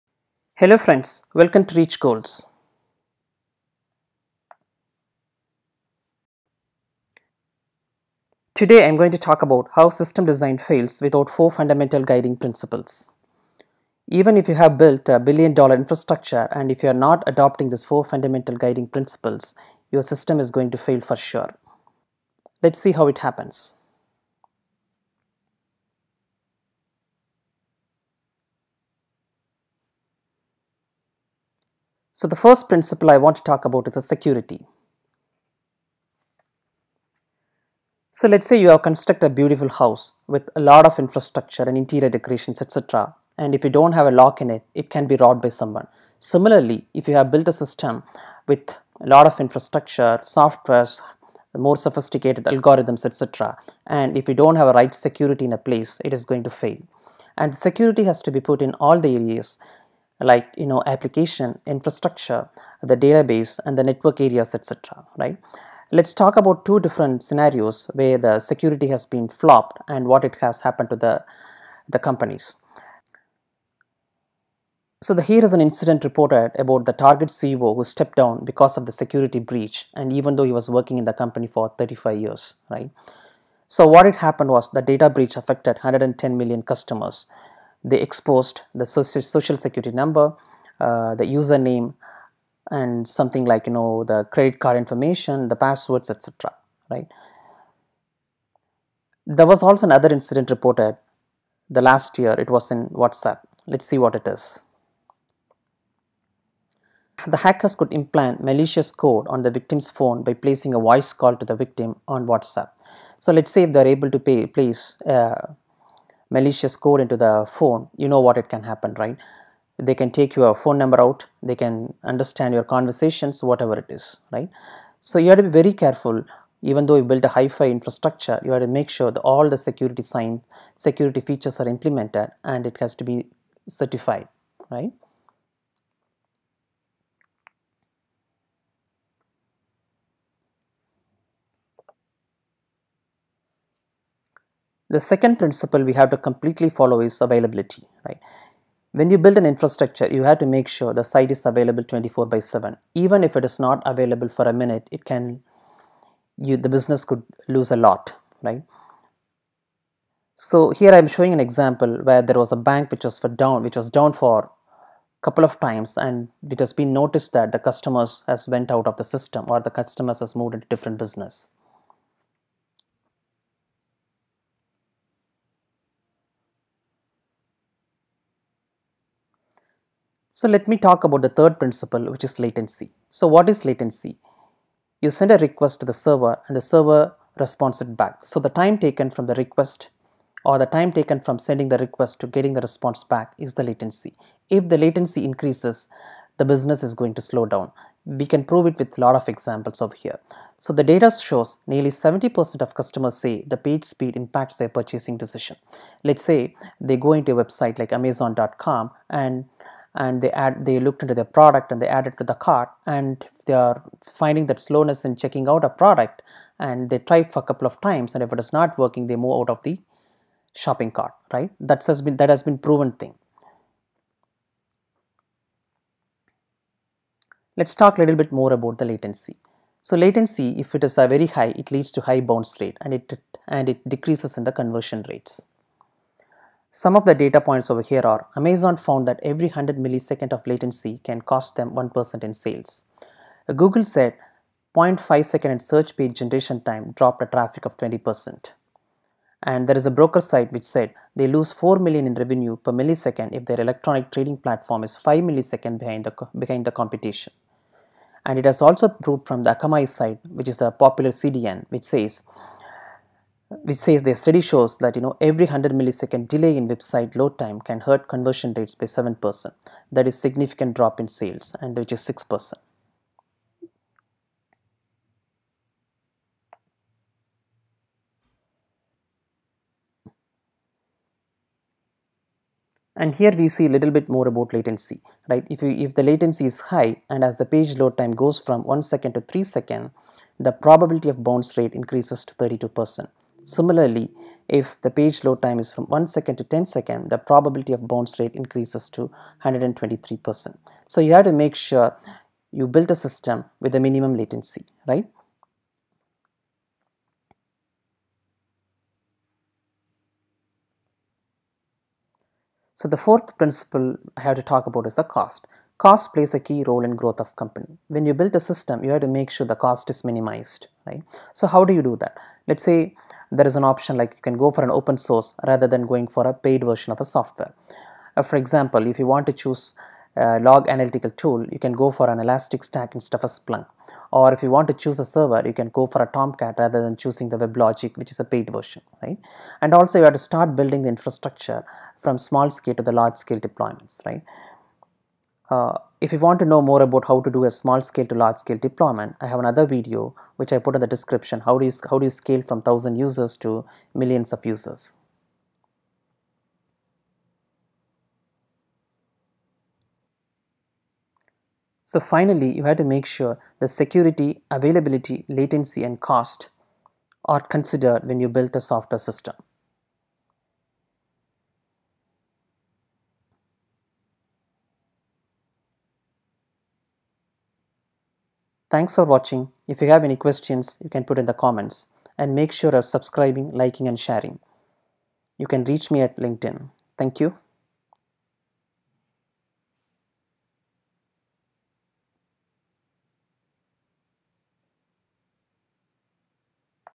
System design fails without 4 fundamental guiding principles
It's true that system design fails without these 4 guiding principles and the system designers should make sure the principles are taken care while designing.

It is also must to discuss this system design principles when you answer System design interview questions.

 1) I have talked about 4 guiding principles with examples.
 2) What will be the impact if the principles are not followed with examples.

Please check how to scale the system from 1000 users.